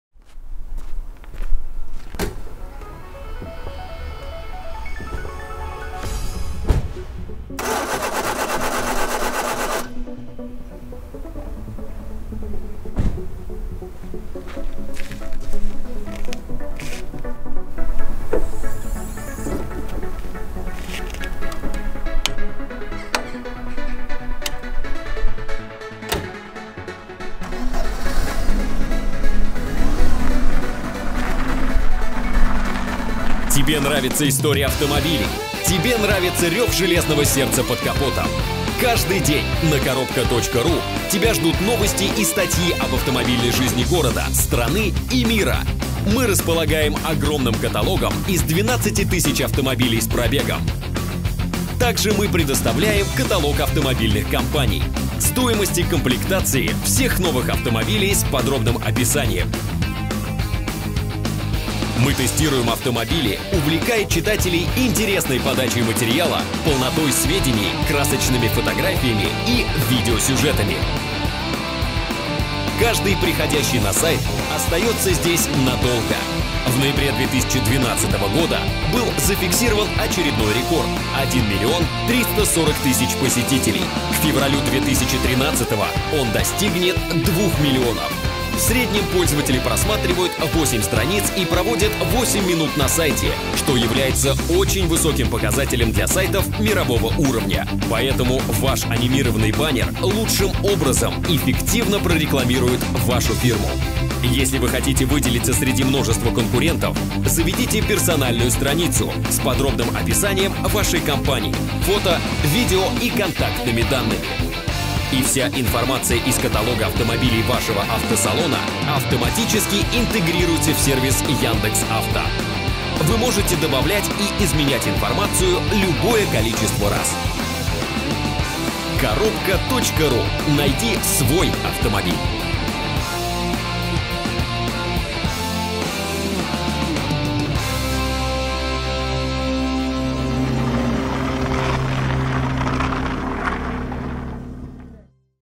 Презентация портала Carobka.ru © "KINOKRUT"
Презентация возможностей портала проходила в кинотеатре "IMAX" в формате 3d!
заказчик: Directline
автор идеи: event агентство Perfecto
исполнитель: творческое объединение "Кинокрут"